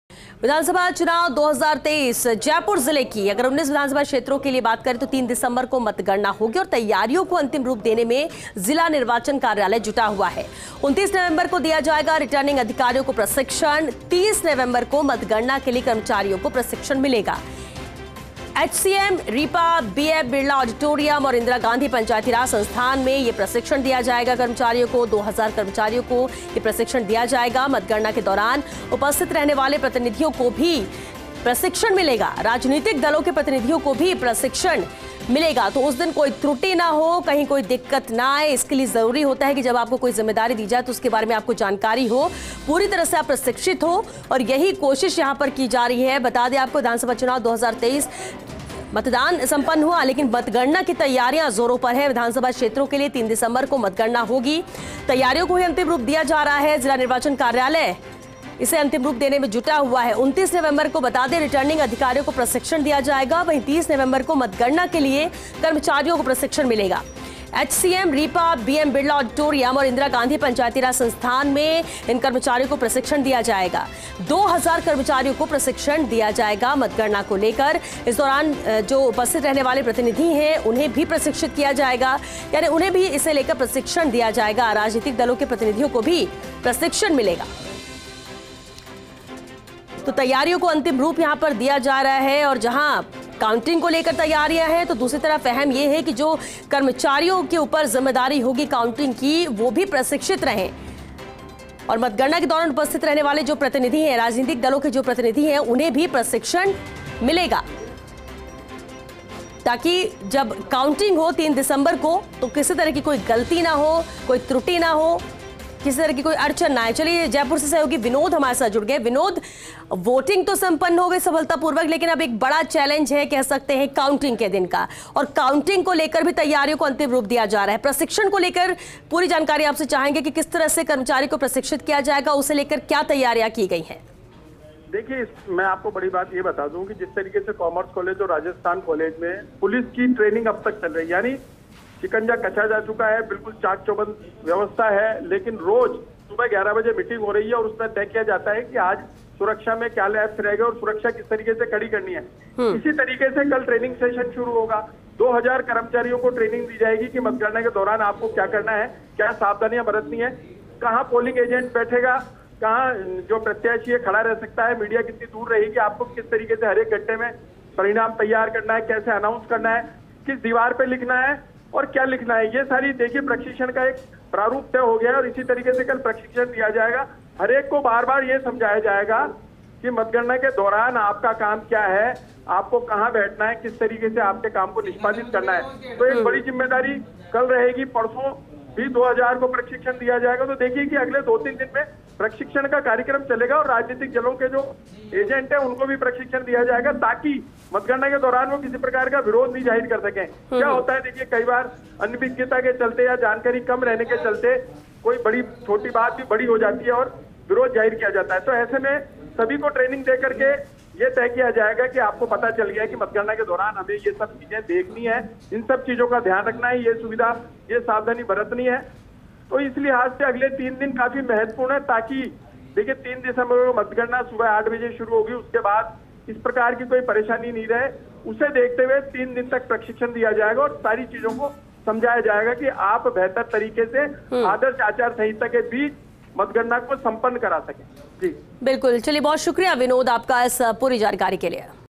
3 दिसंबर को होगी मतगणना, 29 नवंबर को दिया जाएगा रिटर्निंग अधिकारियों को प्रशिक्षण
About this Video:

जयपुर, विधानसभा चुनाव-2023 , जिले के 19 विधानसभा क्षेत्रों के लिए 3 दिसंबर को होगी मतगणना, तैयारियों को अंतिम रूप देने में जुटा है जिला निर्वाचन कार्यालय, 29 नवंबर को दिया जाएगा रिटर्निंग अधिकारियों को प्रशिक्षण, 30 नवंबर को मतगणना के लिए कर्मचारियों को मिलेगा प्रशिक्षण, एचसीएम रीपा, बीएम बिड़ला आडिटोरियम और
इंदिरा गांधी पंचायती राज संस्थान में दिया जाएगा प्रशिक्षण, 2 हजार कर्मचारियों को दिया जाएगा प्रशिक्षण , मतगणना के दौरान उपस्थित रहने वाले प्रतिनिधियों को भी मिलेगा प्रशिक्षण, राजनीतिक दलों के प्रतिनिधियों को भी मिलेगा प्रशिक्षण  | 28  November 2023